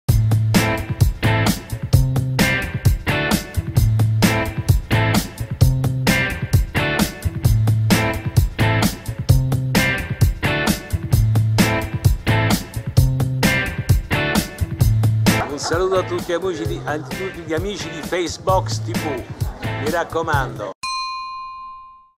Carlo Verdone Greetings to faceBox TV "Italian"
Carlo Verdone Italina film Director and Actor
 Greetings to faceBox TV "Italian"